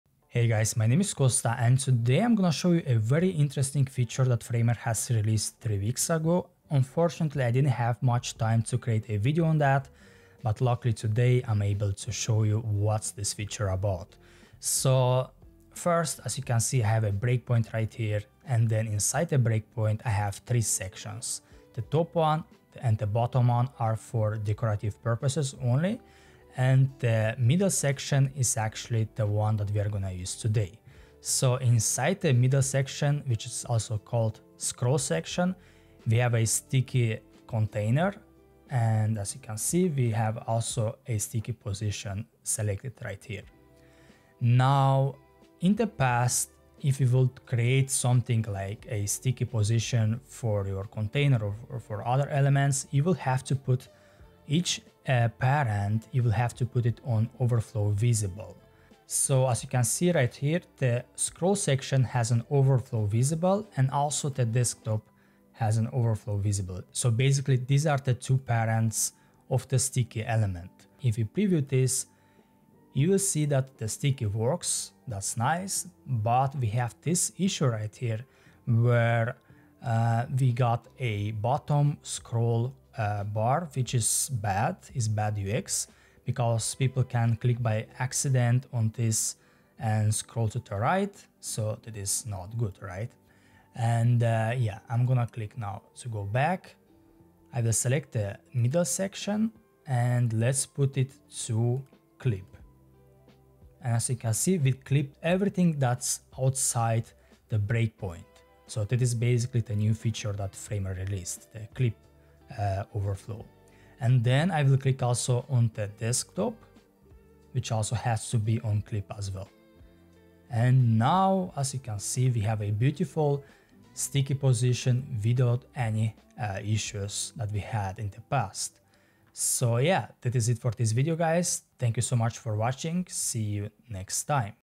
A new way of creating sticky sections in Framer
In this video, I'm gonna show a feature that Framer brought 3 weeks ago. With this new feature you'll be able to create sticky and and fancy scroll transform sections much faster and without the horizontal-bottom bar bothering you.